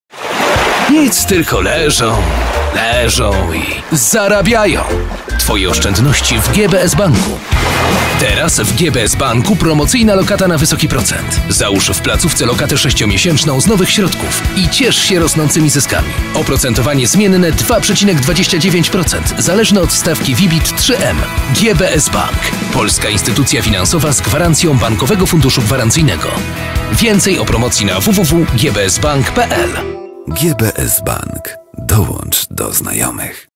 Promocyjna lokata na wysoki procent!
Lokata 6-miesięczna to wygodny sposób na osiągnięcie zysków. To połączenie najlepszego na rynku oprocentowania oraz bezpieczeństwa Twoich pieniędzy!